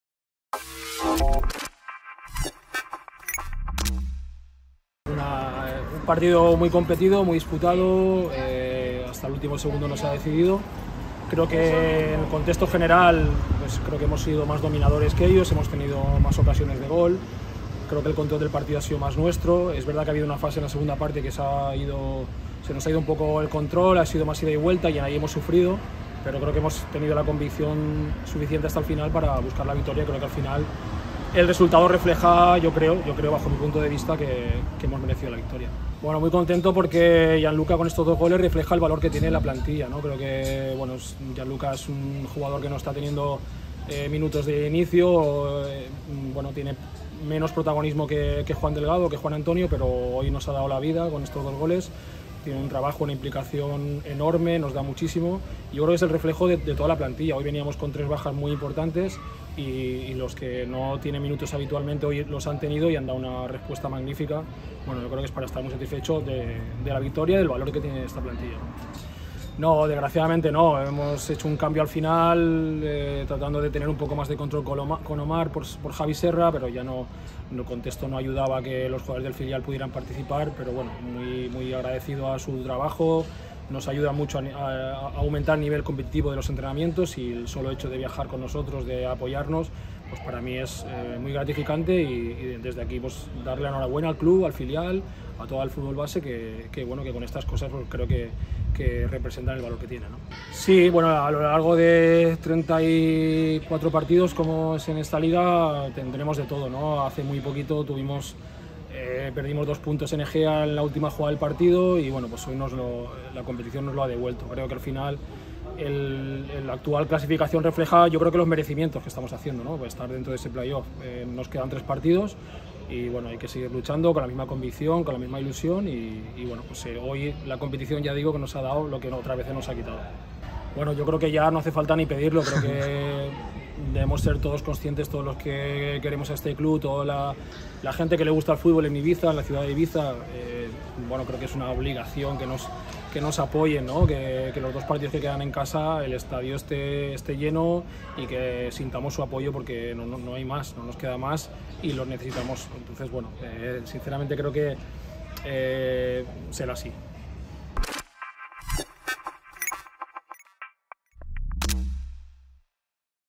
Post Partido J31 AE Prat
Raúl Garrido responde a las preguntas después de la victoria a domicilio frente al Prat, que devuelve al Ibiza a puestos de playoff a falta de 3 jornadas.